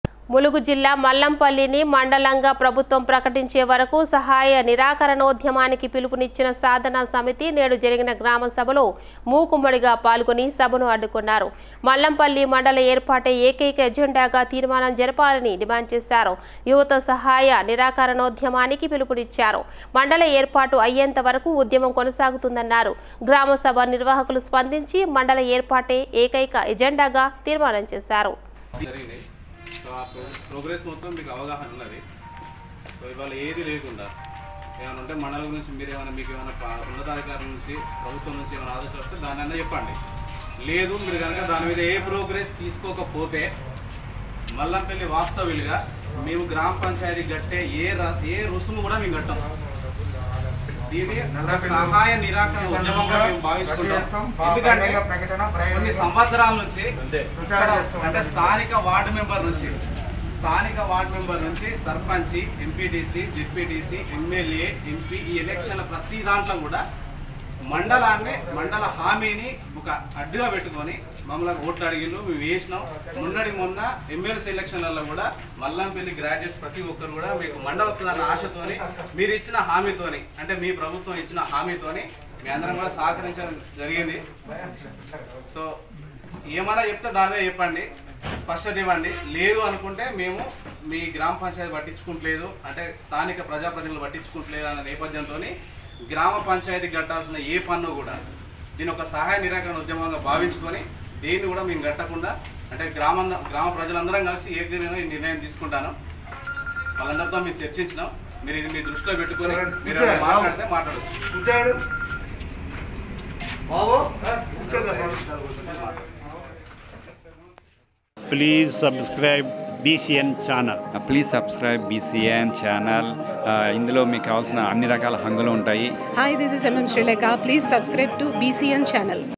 మల్లంపల్లి ని మండలంగా ప్రభుత్వం ప్రకటించే వరకు సహాయ నిరాకరణోద్యమం ములుగు BCN Telugu News
మల్లంపల్లి ని మండలంగా ప్రభుత్వం ప్రకటించే వరకు సహాయ నిరాకరణోద్యమం ములుగు